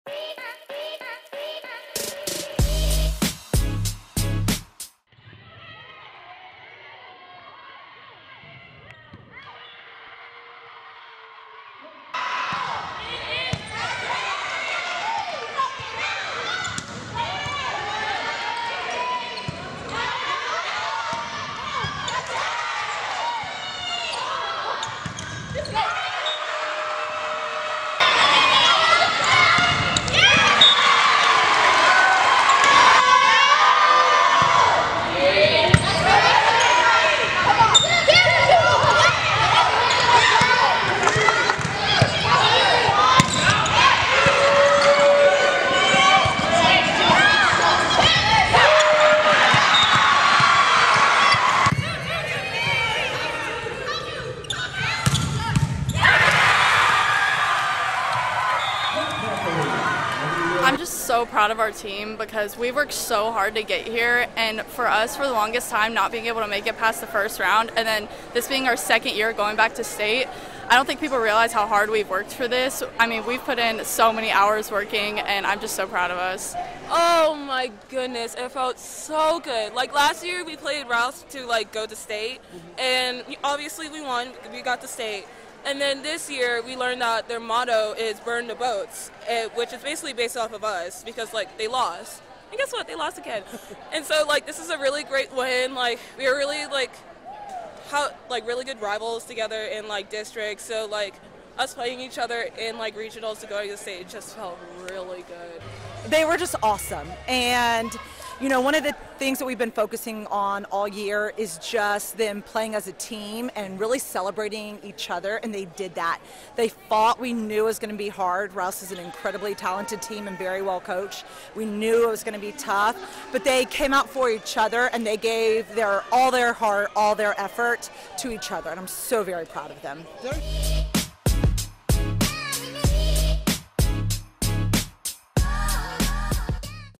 Liberty Hill Rouse Regional Final Highlights
Highlights from the Class 5A-Region IV final between the Rouse Raiders and the Liberty Hill Panthers at Northside Gymnasium in San Antonio on Saturday, Nov. 11, 2023.